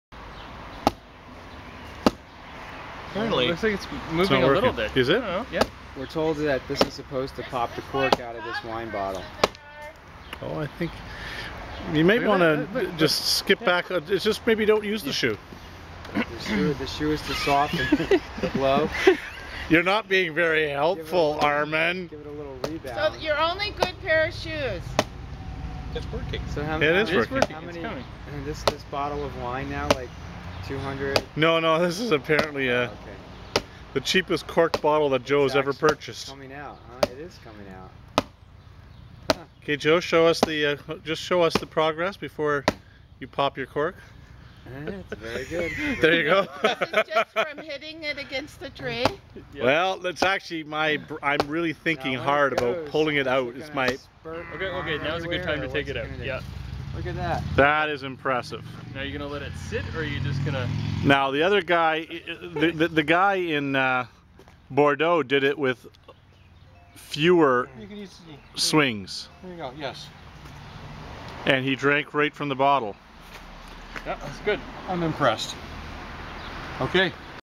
Joe Wine open - Computer.m4v
Joe Lstiburek demonstrates how to open a corked bottle of wine with no tools.